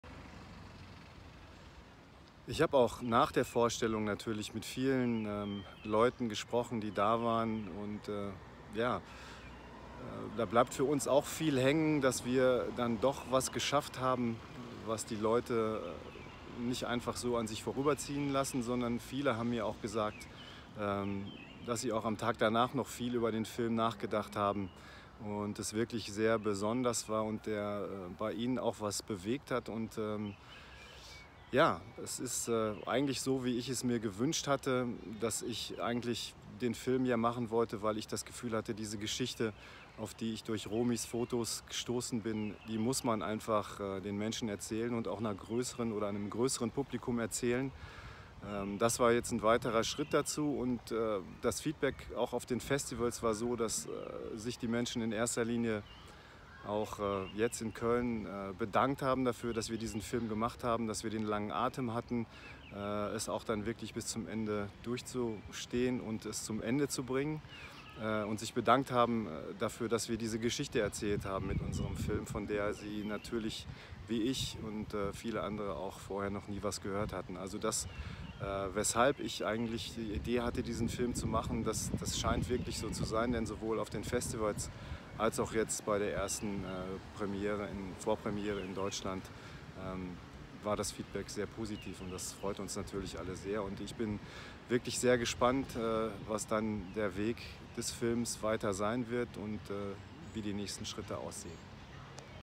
Michael Niermann persönliches Statement zur Exclusiven Preview
Persönliches Satement von Michael (Director) zur Exclusiven Preview am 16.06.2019